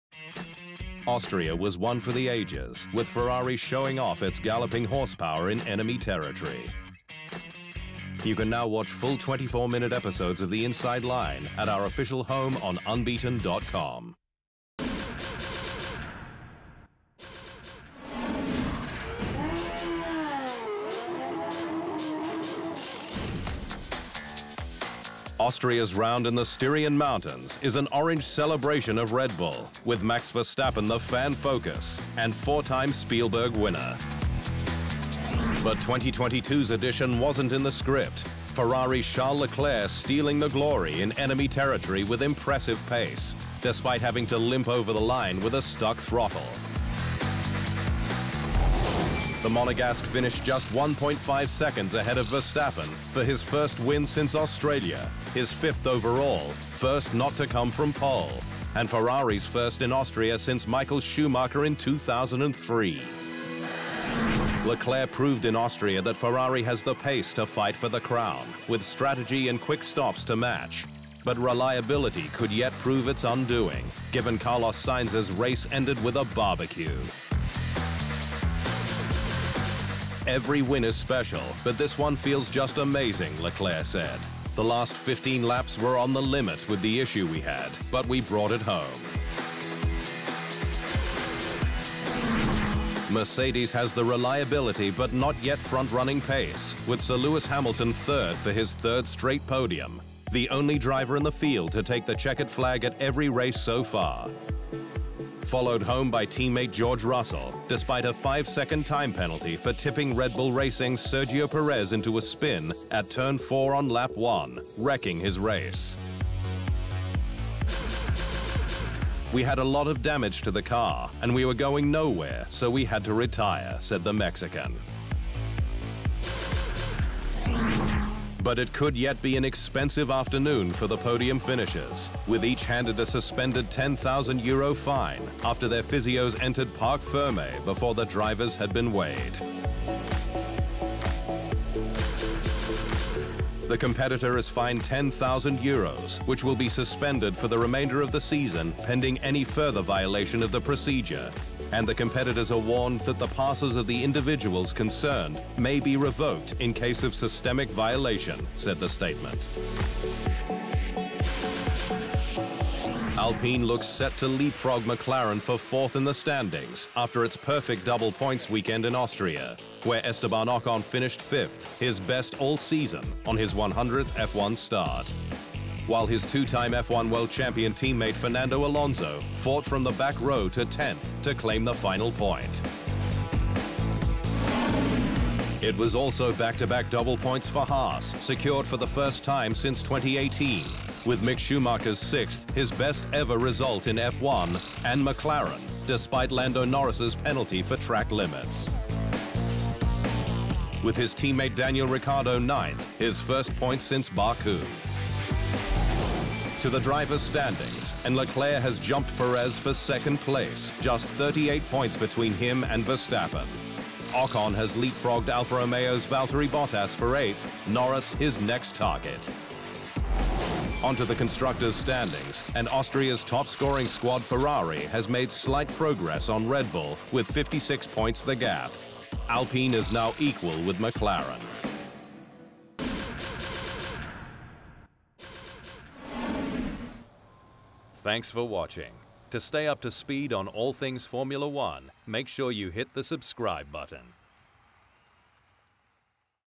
LECLERC PULLED THROUGH: 2022 AustrianGP Wrap
Austria’s round in the Styrian mountains is an orange celebration of Red Bull, with Max Verstappen the fan focus, and four-time Spielberg winner.

But, 2022’s edition wasn’t in the script, Ferrari’s Charles Leclerc stealing the glory in enemy territory with impressive pace, despite having to limp over the line with a stuck throttle.

Follow @unbeatenf1 on Facebook, Twitter and Instagram.